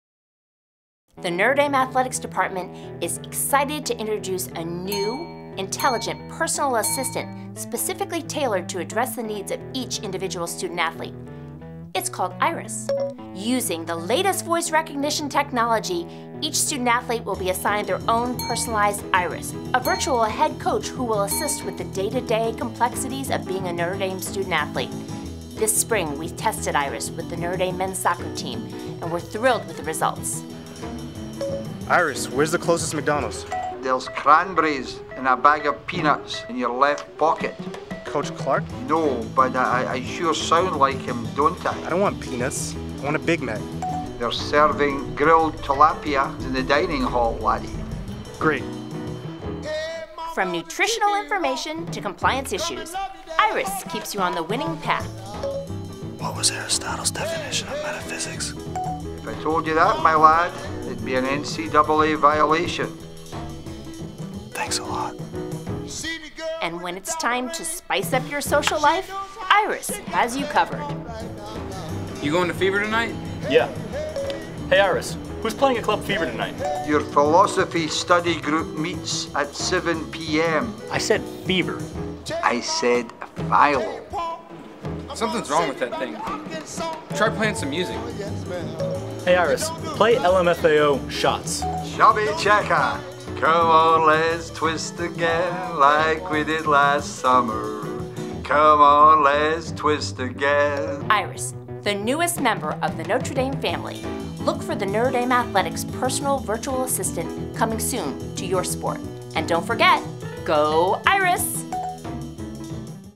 Meet IRIS - the New Notre Dame Athletics Personal Assistant
The Notre Dame Department of Athletics held its annual O.S.C.A.R.S. (Outstanding Student-Athletes Celebrating Achievements & Recognition Showcase) on Wednesday evening, May 2, 2012. A yearly tradition at the event is the creation of parody 'commercials' for the athletic department - here Notre Dame 'introduces' IRIS - a 'New Intelligent Personal Assistant.' The voice actor in the video is Notre Dame head soccer coach Bobby Clark.